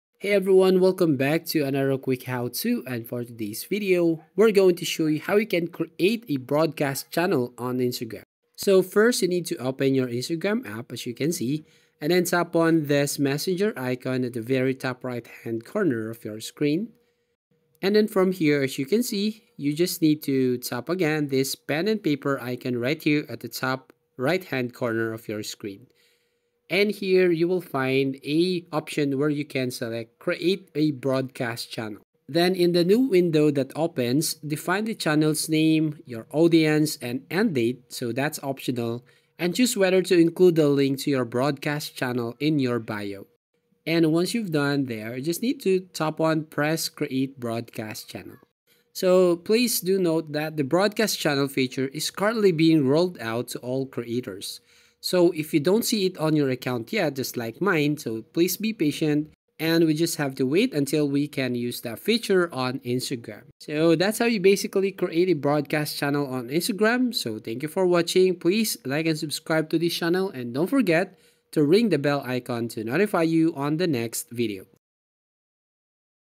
How To Create a Broadcast Channel on Instagram (NEW UPDATE)
In this video you will learn how to create a broadcast channel on Instagram.